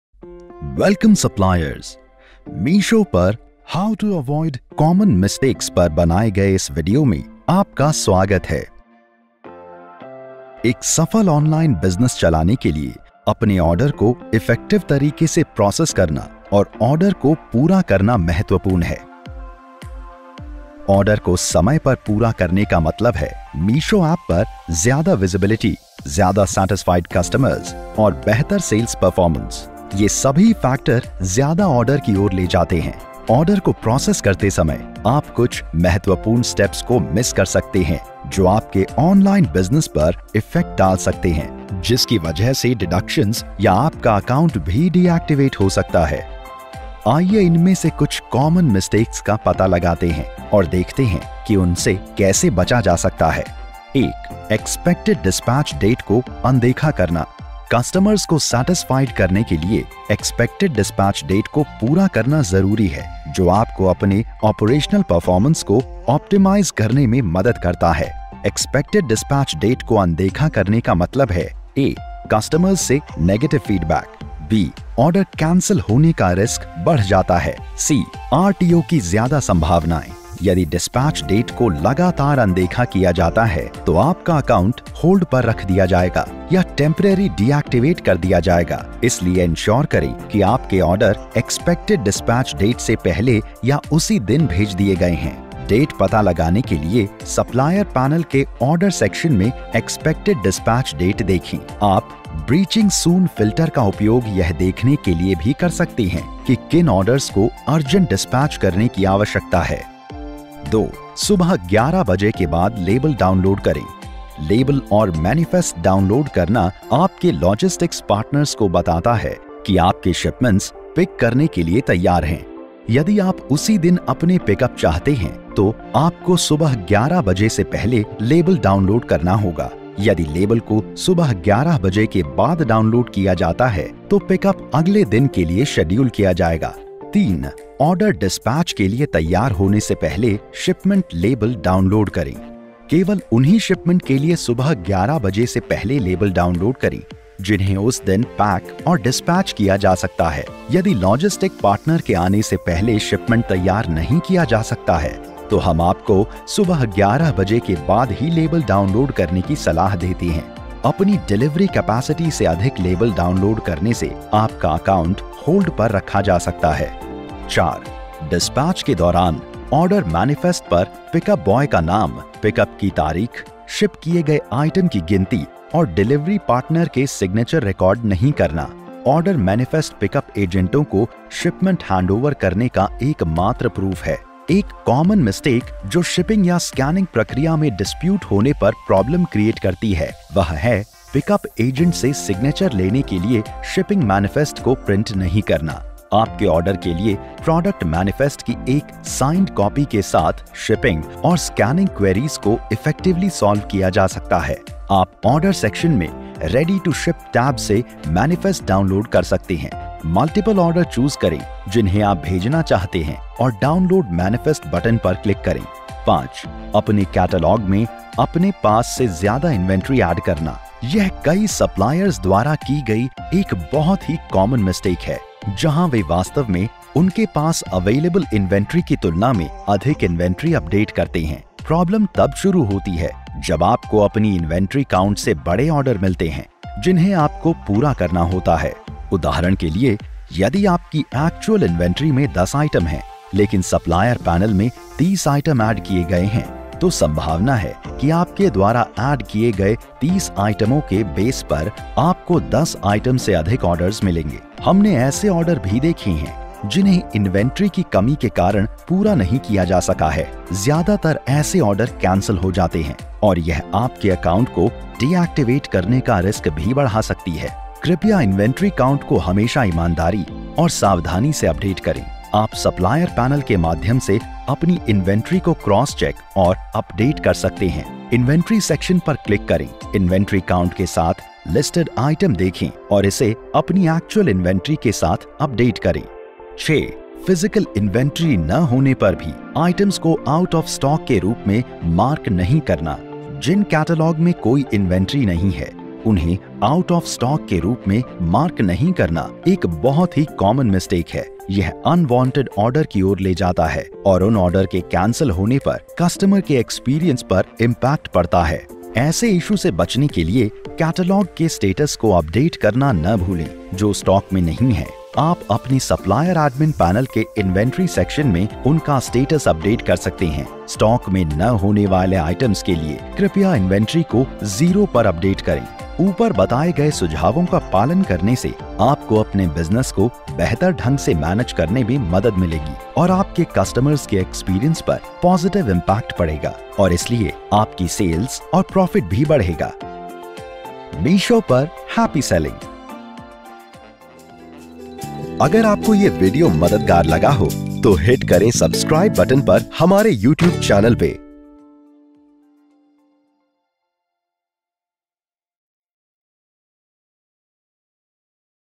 ✔ Avoid Kare Common Seller Mistakes | How to Reduce Loss | Order Returns, RTO & Charges on Meesho
Ek successful online business chalane ke liye apne orders ko effectively process karna aur pura karna bahut jaruri hai. Apke business ko smoothly chalane ke kuch benefits me, Meesho app par higher visibility, satisfied customers aur better sales shamil hai. Order management issue ke karan aapse charge liya ja sakta hai.

0:00 Introduction
0:54 Ignoring expected dispatch date
2:00 Downloading labels after 11AM
2:27 Downloading labels before order is ready for dispatch
2:57 Not recording details on order manifest
3:46 Adding incorrect inventory
5:15 Not marking out of stock items

Aap kuch common mistakes kar sakte hai jiski wajah se aap ke account ko hold par rakha ja sakta hai ya aapke business par charge lagaye ja sakti hai. Inn common mistakes aur online selling karte time hone wali cheezo se aware rahe. Yahan kuch common mistakes di gayi hai jo naye online seller karte hai aur unse kaise bachna chahiye.

Expected dispatch day ko ignore karna. Iski wajah se customer se negative feedback milta hai, order cancel hone ka risk badh jata hai aur rto ka chance bhi badh jata hai.

Yadi dispatch date ko lagatar ignore kiya jata hai to aap ka account hold par kar diya jayega ya temporarily deactivate kar diya jayega. Date find karne ke liye Supplier Panel ke ”Order” section me expected dispatch date check kare. Aap “Breaching Soon” filter ka upyog yah dekhne ke liye bhi kar sakte hai ki kin orders ko urgent dispatch karne ki jarurat hai.

Subhe 11 bje ke baad shipping label download kare, label download karna hume ye notify karta hai ki apka order dispatch ke liye ready hai. Yadi aap same day apne pickup chahte hai to aap ko subhe 11 baje se pahle label download karna hoga. Aur agar ise subhe 11 baje ke baad download kiya jata hai to pick up agle din ke liye schedule ki jayegi.

Dispatch ke liye order ready hone se pahle shipping label download kare. Keval unhi shipment ke liye subhe 11 baje se pehle label download karna ensure kare jinhe usi din pack aur dispatch kiya ja sakta hai. Shipment ke dispatch ke liye ready hone se pahle label download karne se apka account hold par rakha ja sakta hai.

Dispatch ke time order manifest par pickup boy ka naam, pickup ki date, ship kiye gaye items ki counting, aur delivery partner ke signature ko record nahi karna: order manifest hi ek aisa proof hai ki apka shipment pickup agent ko de diya gya hai aur dispute hone par reference ke liye jaruri hota hai. Pickup agent ke aap tak pahuchne se pahle ‘Order’ section me ‘Ready to Ship’ tab me order ke manifest download kare aur print kare.

Apne catalog me available inventory se jyada inventory add karna: iske karan apko aise order received ho sakte hai jinhe aap pura nahi kar sakte. Cancelled ya auto-cancelled orders ka matlab hai ki apke account ka deactivate hone ka risk hai. Apni inventory ko update karna ensure kare aur ise daily cross check kare.

Jab aapke paas inventory khatam ho jati hai to item ko out of stock mark nahi karna: aap supplier panel me jaaker ‘inventory’ par click karke aur product ki ‘inventory’ ko zero par update karke aise products ke liye inventory ko update kar sakte hai.

Ab jab aap jaan gaye hai ki aesi mistakes se kaise bachna chahiye to aaj hi bina kisi tension ke Meesho par selling karna shuru kare.

Happy Selling on Meesho